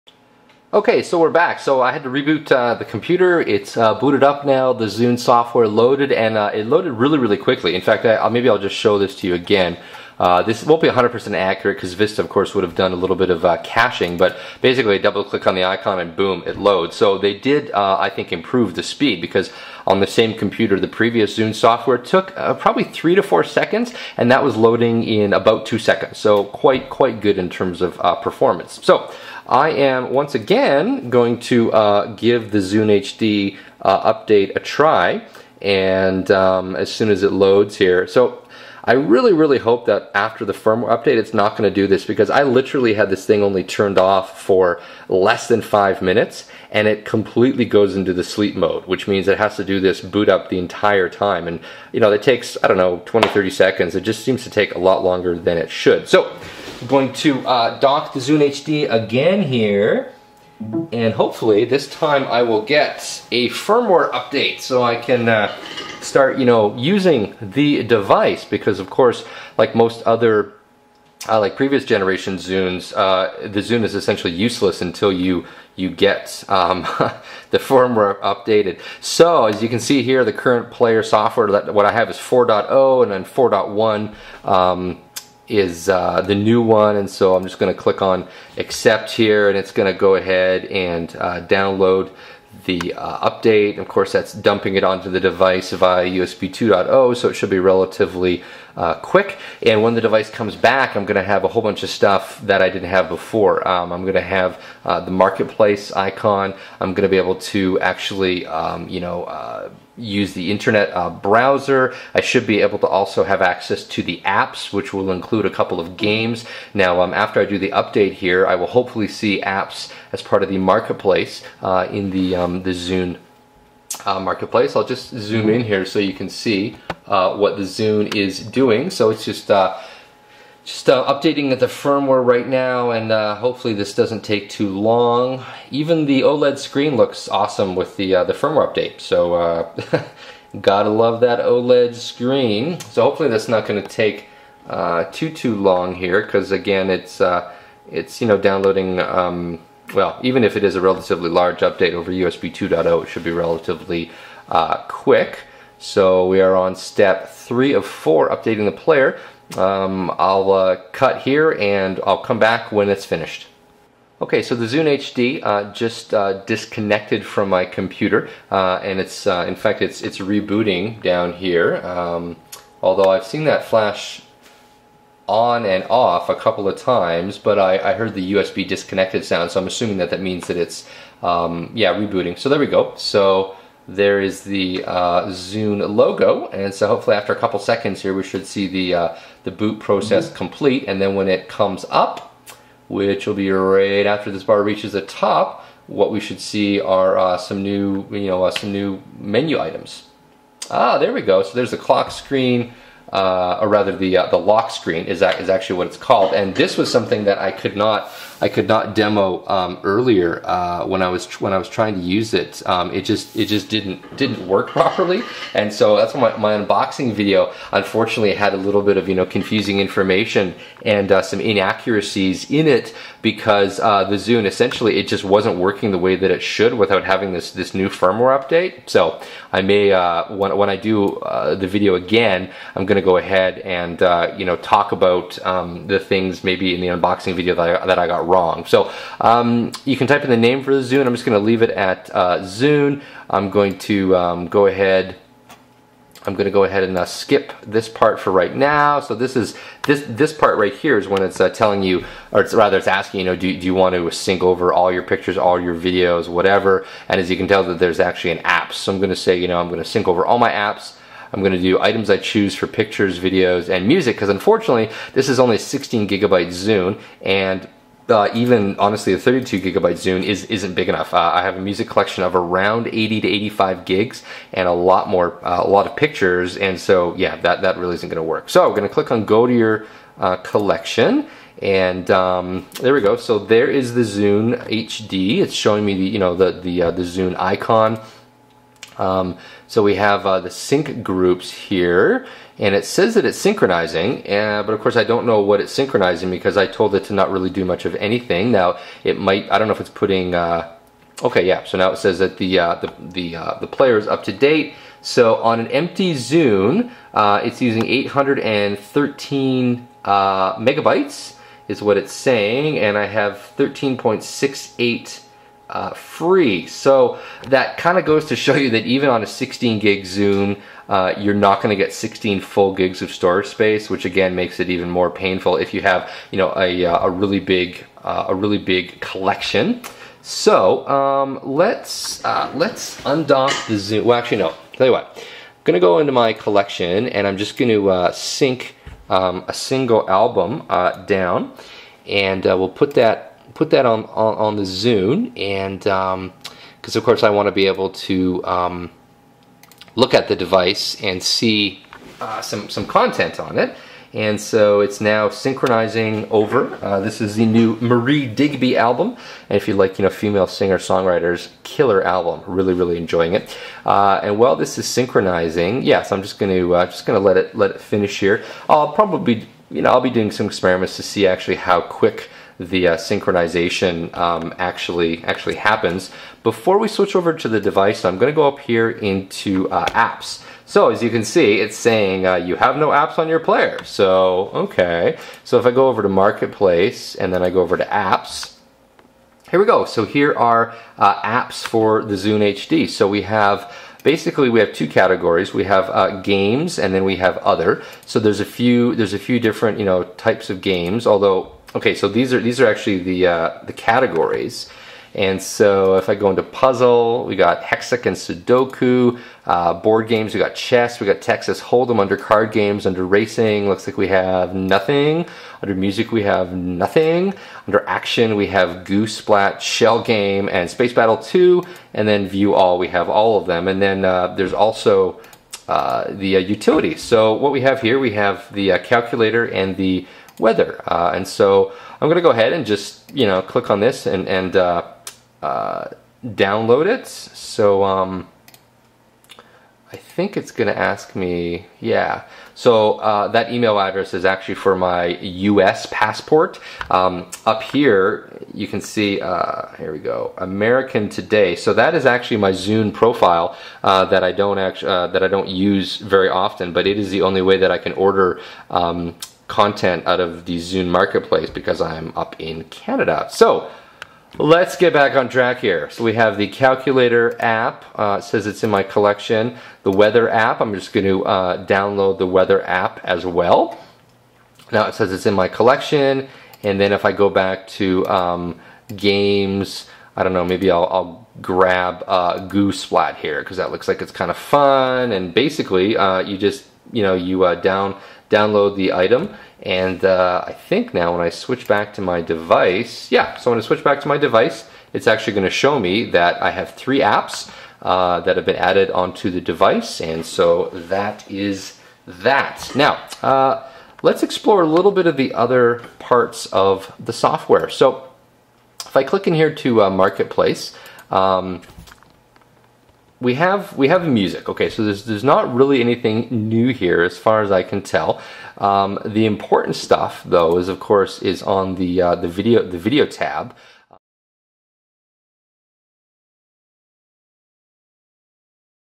DEEP DIVE: Zune 4.0 Software (Part 2)
This is a "deep dive" video where I take a closer look at the Zune 4.0 desktop software. This is part two of three.